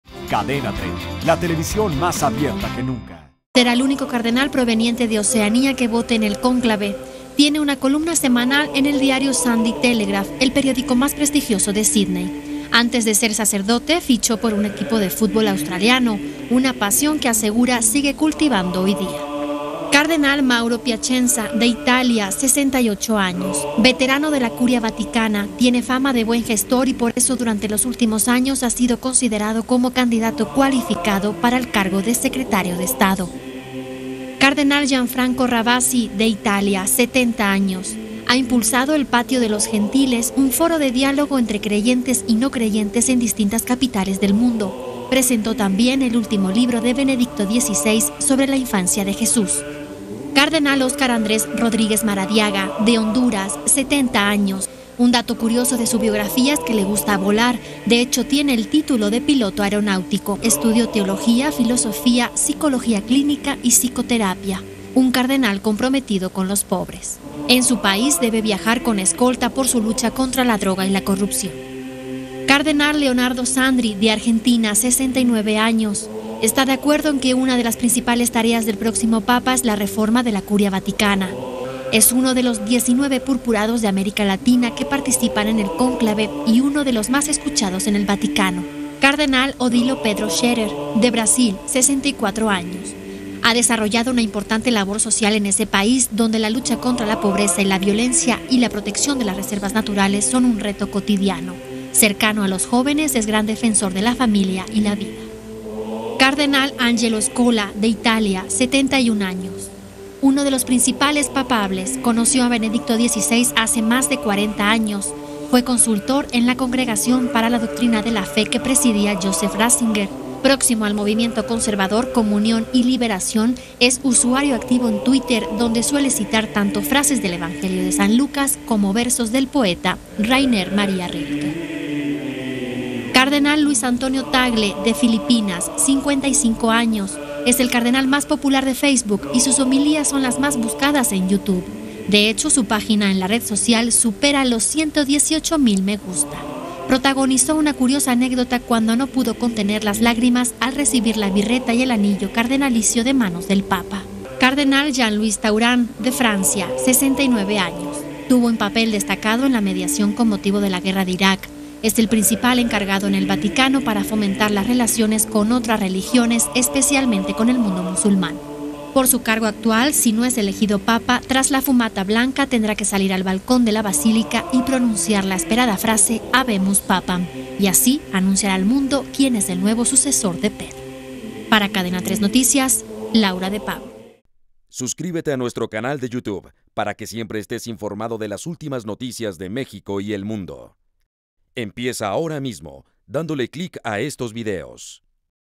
Los candidatos a suceder a Benedicto XVI (Parte 2)
Los candidatos que más suenan para ocupar la silla de San Pedro son el australiano George Pell, el italiano Mauro Piacenza, el hondureño Oscar Andrés Rodríguez, el argentino Leonardo Sandri y el brasileño Odilo Pedro Scherer.